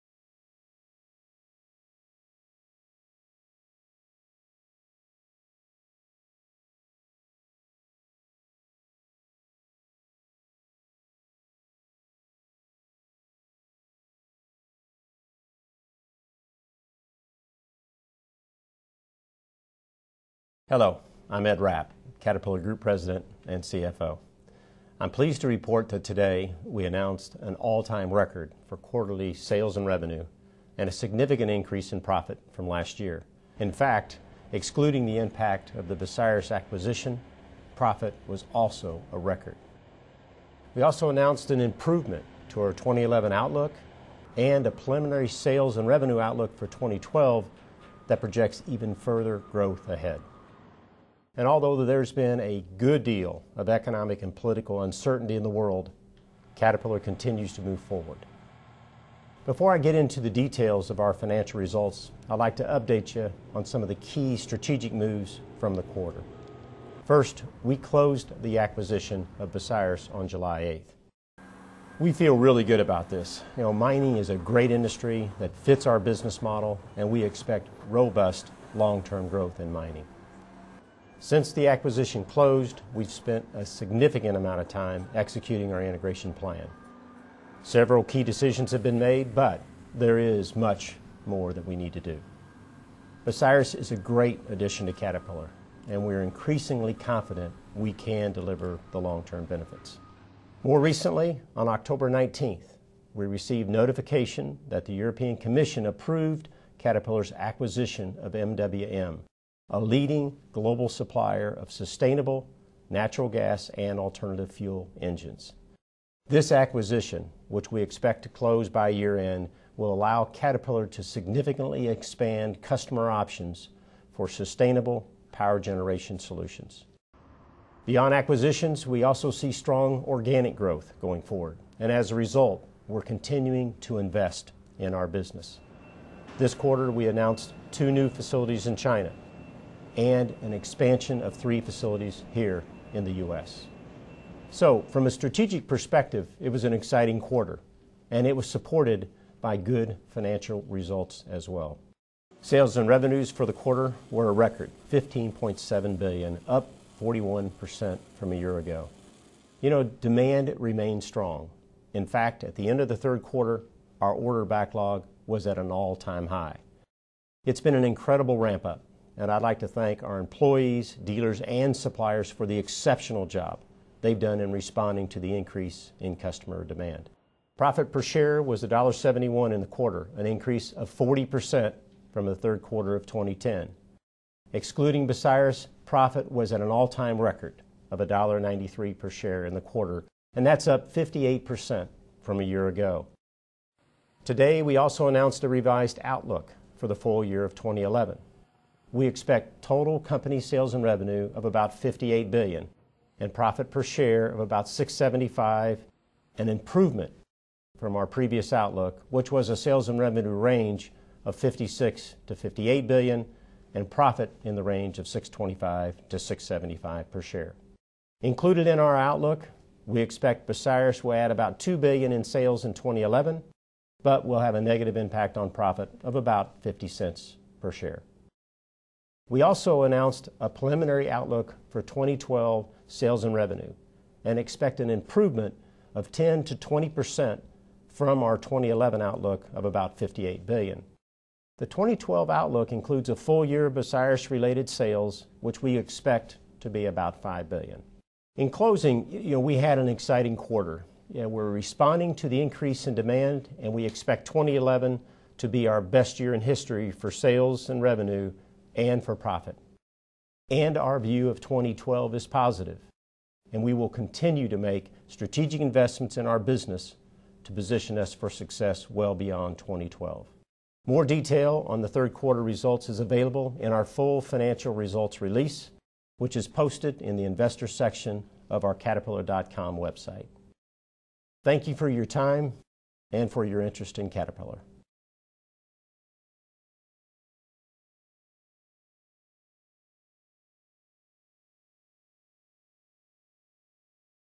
Caterpillar Financial Results | Q3 2011 | Record Sales & Revenues
View a video overview of quarterly results, for the third quarter of 2011, provided by Caterpillar Group President and Chief Financial Officer Ed Rapp.